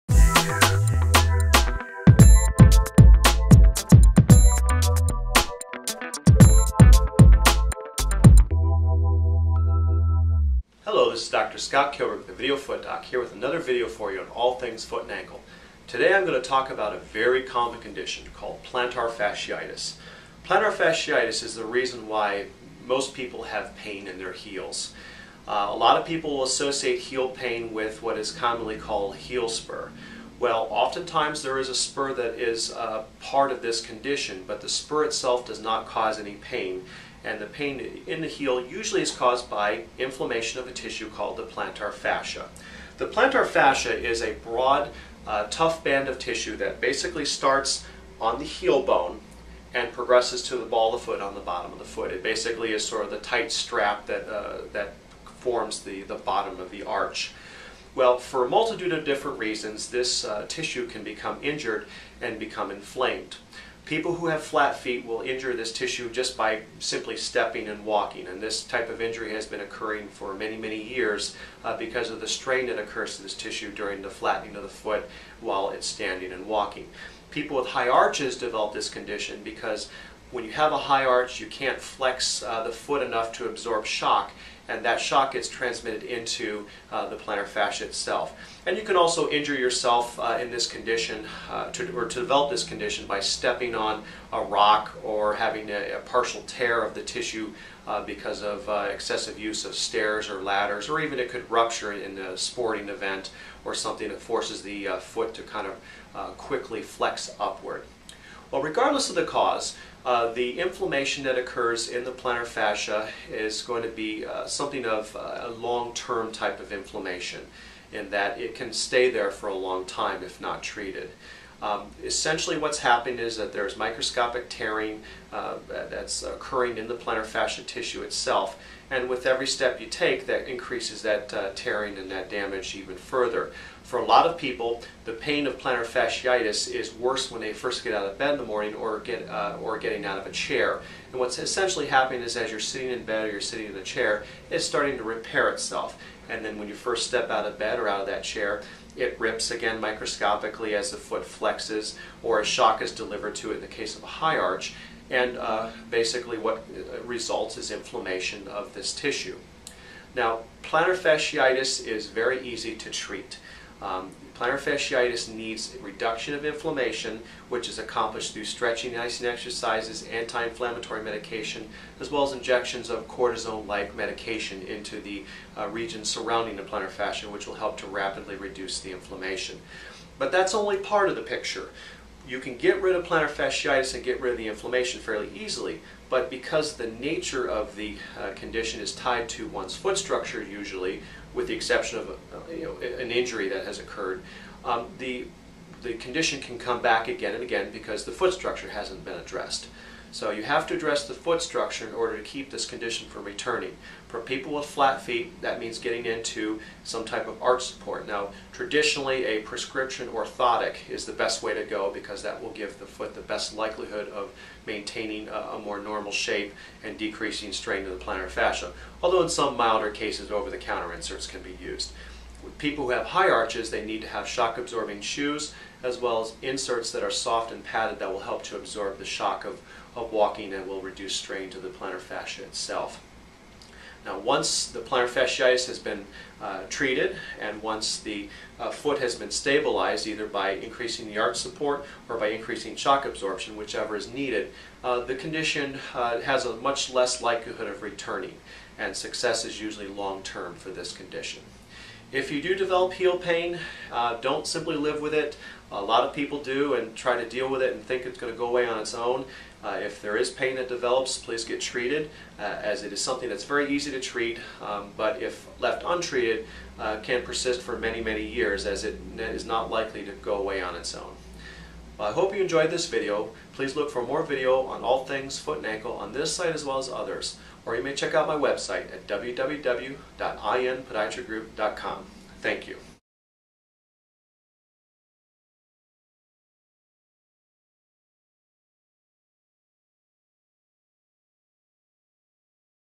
Plantar Fasciitis
This video discusses the common cause of heel pain, plantar fasciitis.  The factors that lead to this injury are discussed, as well as the treatment options available.